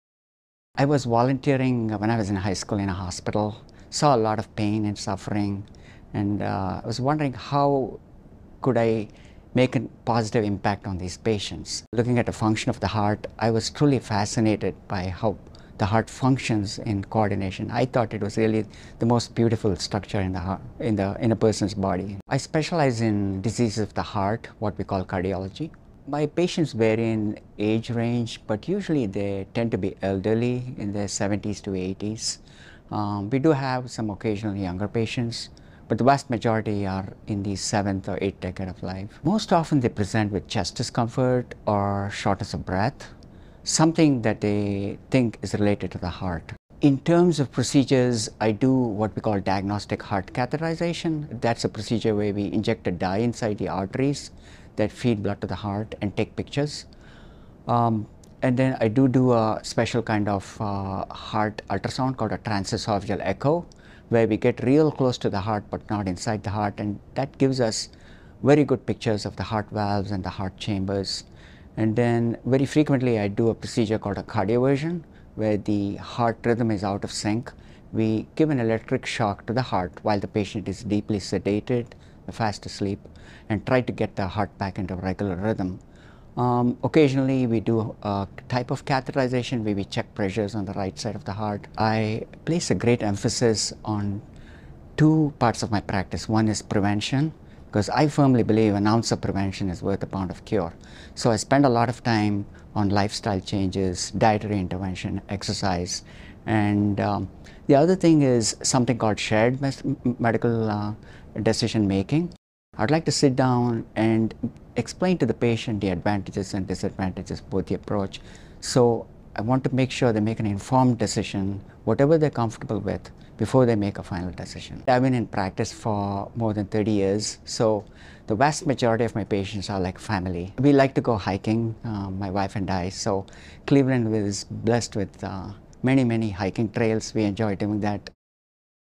Khodanpur Guruprasad, MD | Cleveland Clinic Cardiovascular Medicine
Cardiologist Khodanpur Guruprasad, MD, ensures that his patients fully comprehend their condition and treatment options, allowing them to make informed decisions as a team. Dr. Guruprasad specializes in the management of heart disease, including prevention and medical procedures.

We're here when you need us — for every care in the world.